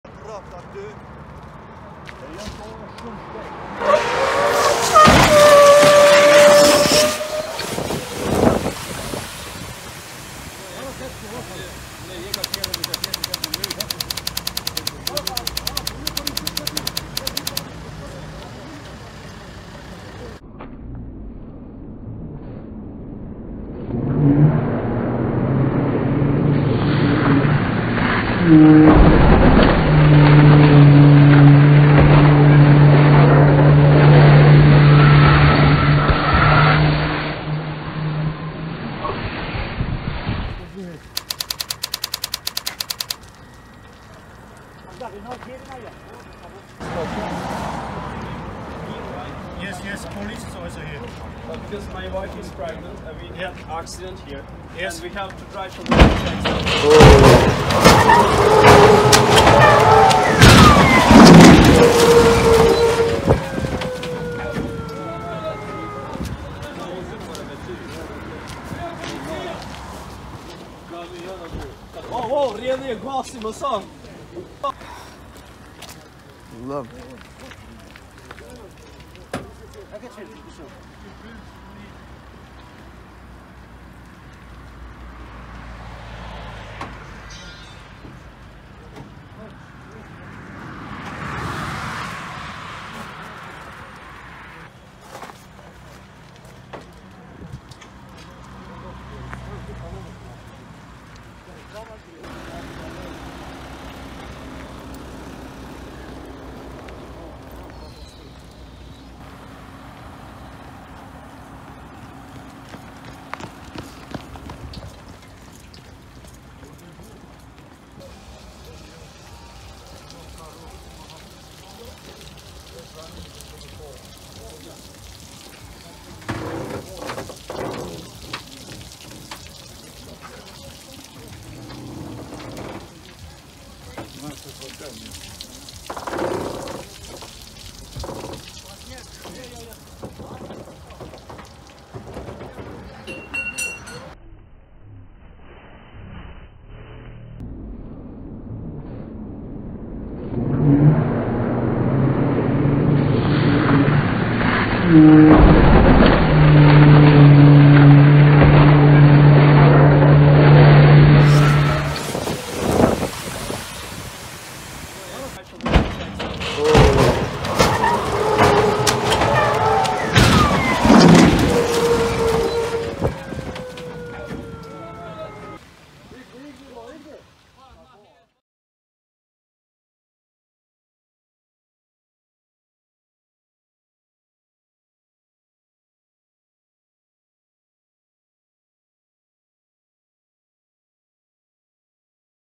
Terrible Cars Accident
A big accident happened as a result of heavy fog on the highway
"Ibrahim Rugova" in Kosovo More than 10 cars were damaged.